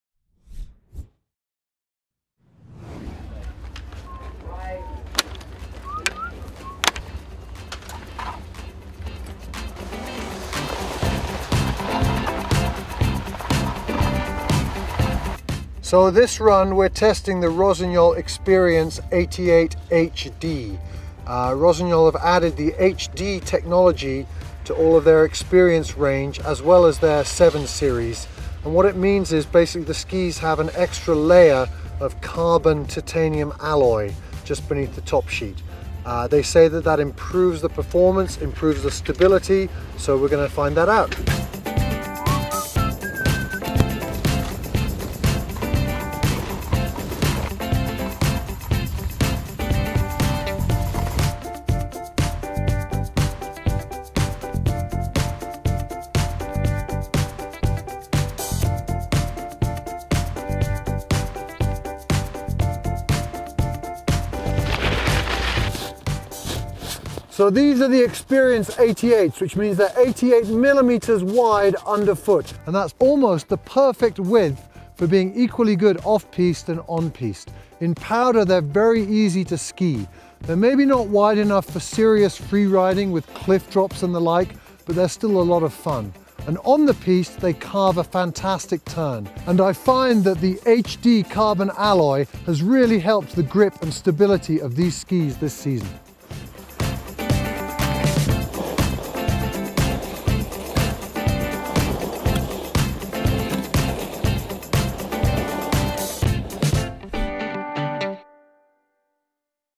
Ski Test Reviews 2016/17: Rossignol Experience 88 HD
Exclusive reviews of this season's new skis from the experts on the annual ski test for Telegraph Ski & Snowboard.

Read the full reviews online at telegraph.co.uk/ski